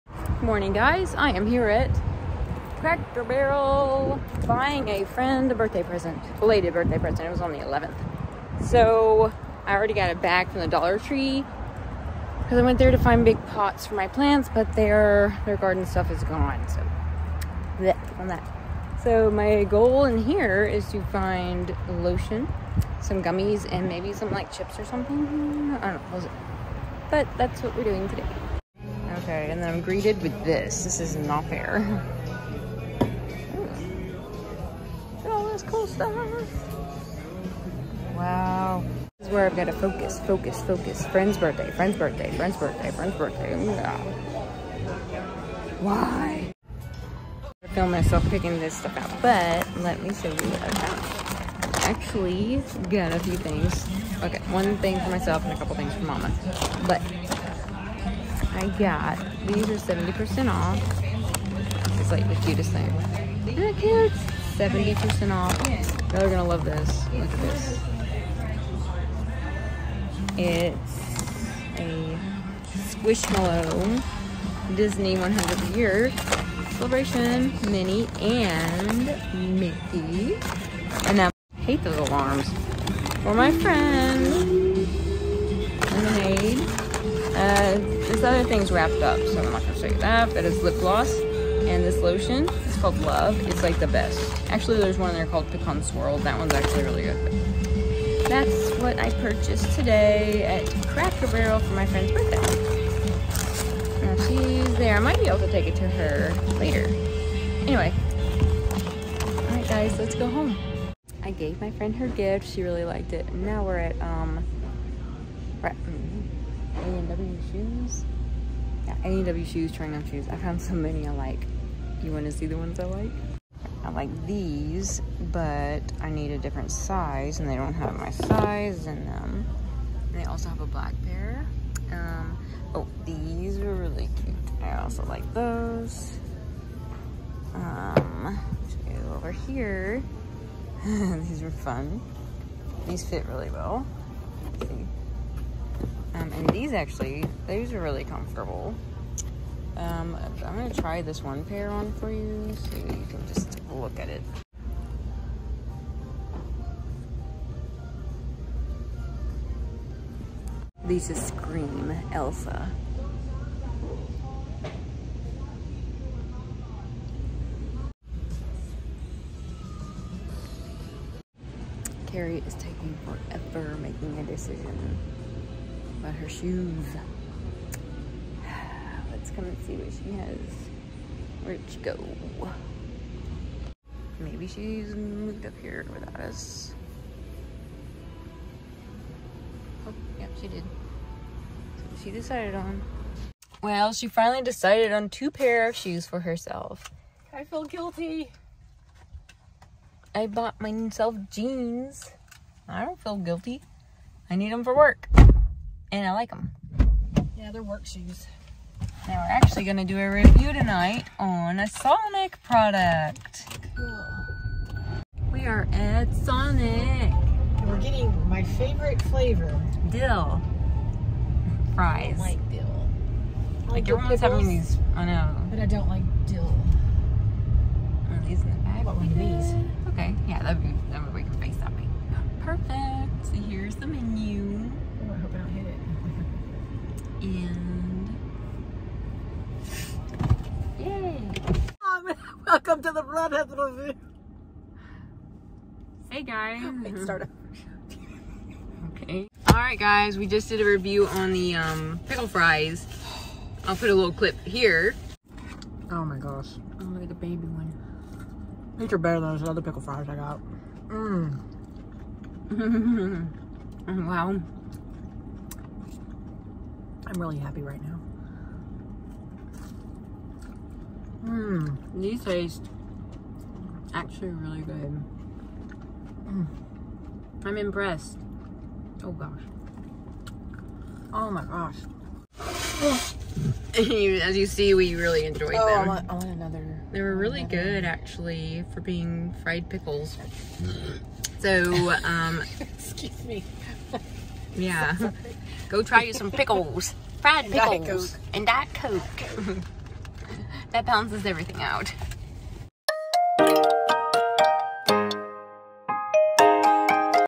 A BIRTHDAY GIFT, SHOES AND PICKLE FRIES OH MY!!
Im glad she liked her gift!!

We went to DSW shoes. I really liked the neon yellow ones. Ha ha!

I can’t believe I liked the pickle fries. They had a really good flavor.